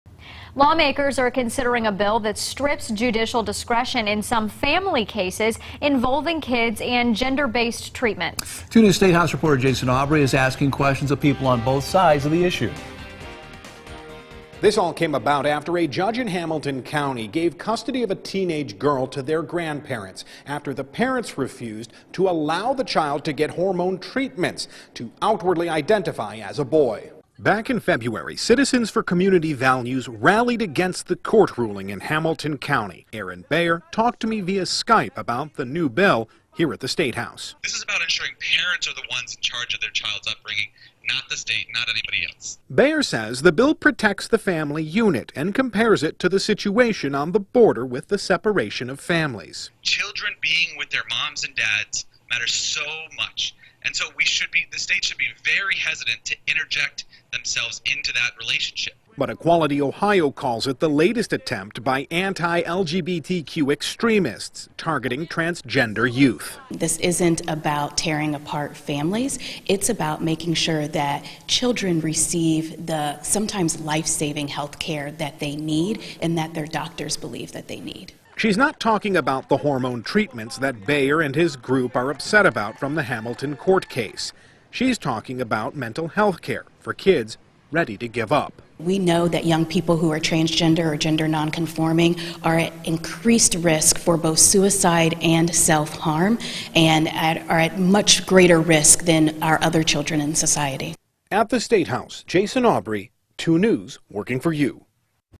Opponents of 'Parent's Rights Bill' say it's unnecessary, discriminatory against transgender youth
This all came about after a judge in Hamilton County granted custody of a teenager girl to their grandparents after the parents refused to allow the child to receive a hormone treatment to identify outwardly as a boy.